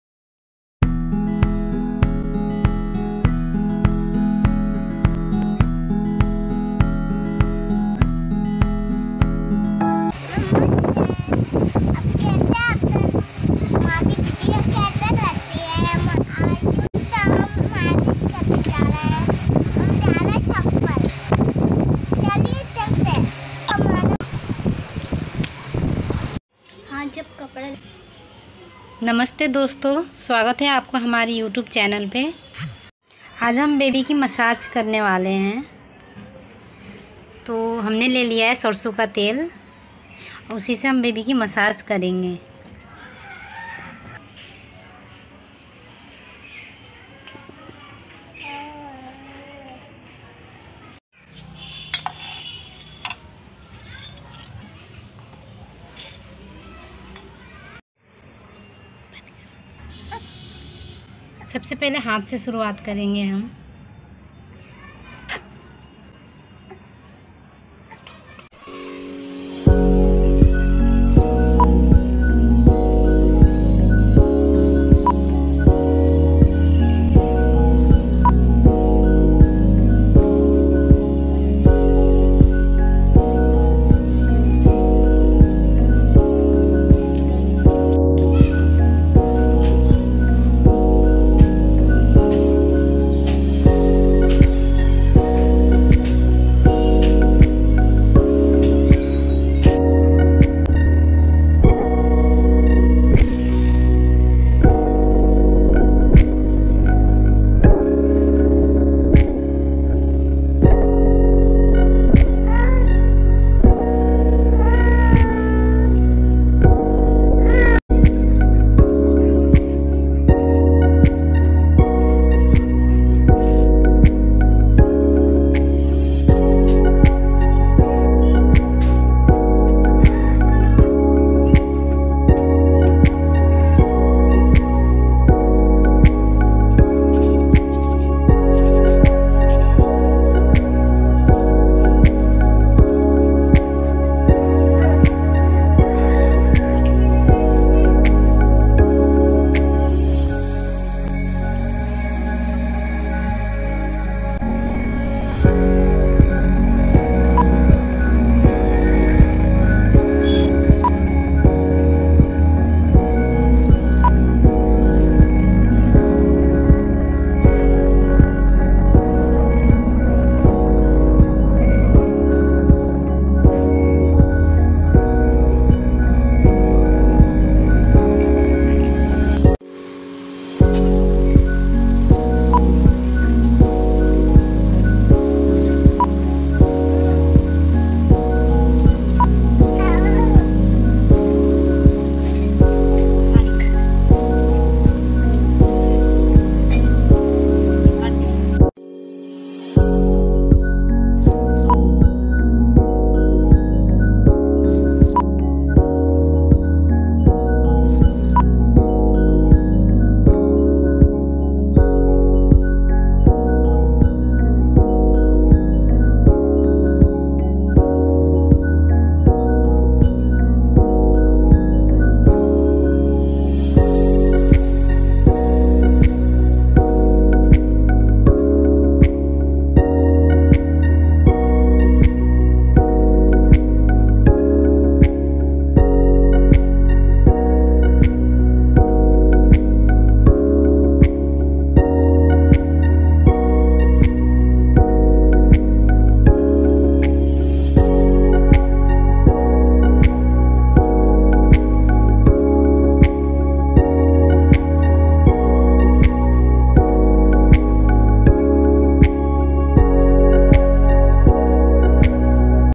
How to massage a baby in Indian styles ❤️7months baby boy ll Step by step 👍
thanku so much for watching this video 🙏👍
Support my new Channel dosto
We Are Happy for your supporting 🥰